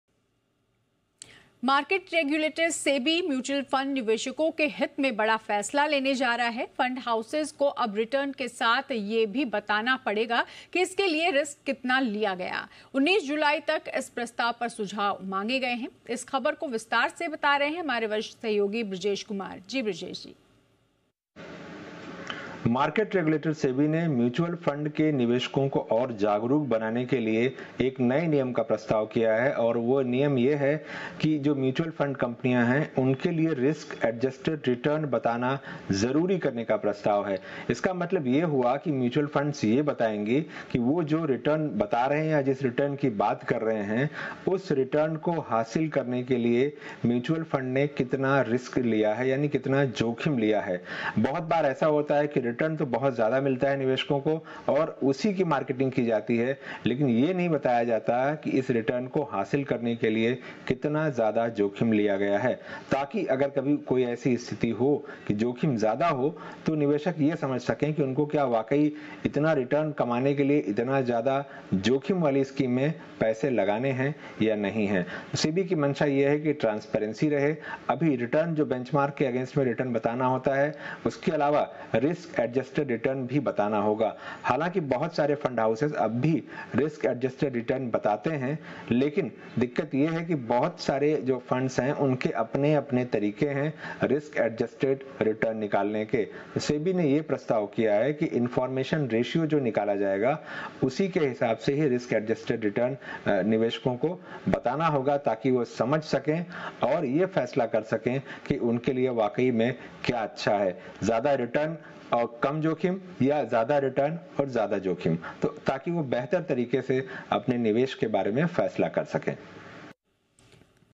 SEBI proposes Mandatory disclosure of risk-adjusted mutual fund returns on the cards
निवेशकों को रिटर्न ही नहीं, रिटर्न के लिए उठाया जोखिम भी बताना होगा, SEBI का म्यूचुअल फंड्स के लिए अहम प्रस्ताव

जानिए पूरी खबर ब्रजेश कुमार से